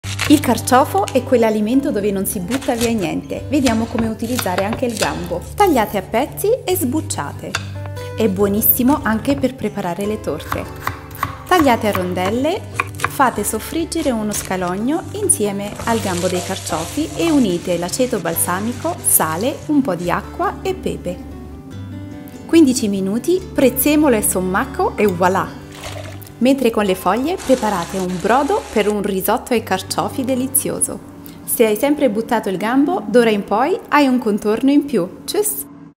Come utilizzare tutto del Carciofo anche i gambi!
Un contorno anti spreco e super buono😋

INGREDIENTI:
•Gambi puliti 500 g
•aceto balsamico 20 ml
•acqua 50 ml
•scalogno 1

PROCEDIMENTO:
•pulite i gambi, sbucciate e tagliate a rondelle
• fate soffriggere uno scalogno e unite i gambi e lasciateli soffriggere  un po’
• aggiungete l'aceto, l'acqua, il sale, il pepe e lasciate cuocere per 15 minuti
• condite con prezzemolo e sommacco
Buon appetito 🤗